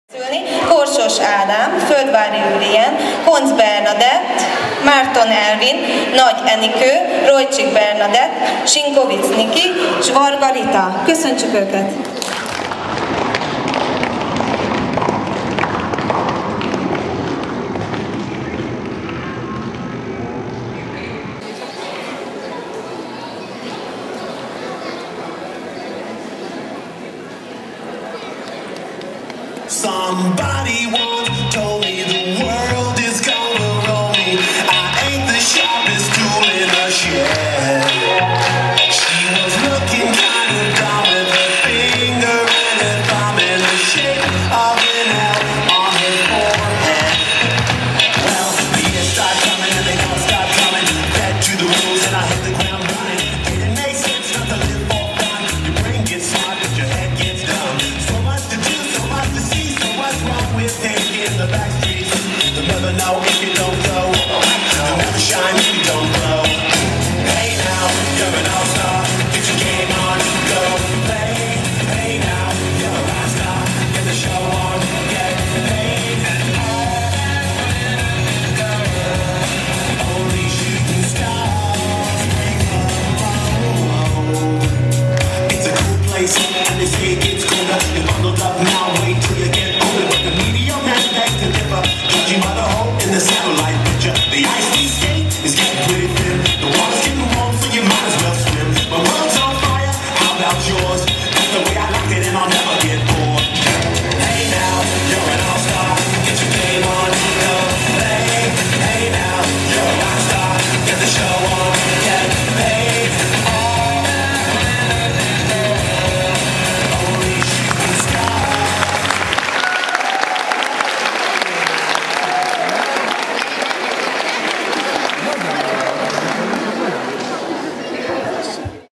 2006.05.21 MONORI VADÓCOK FELLÉPÉS MONOR.mp4
2006. május 21. - a Monori Vadócok első fellépése a monori Piactéren.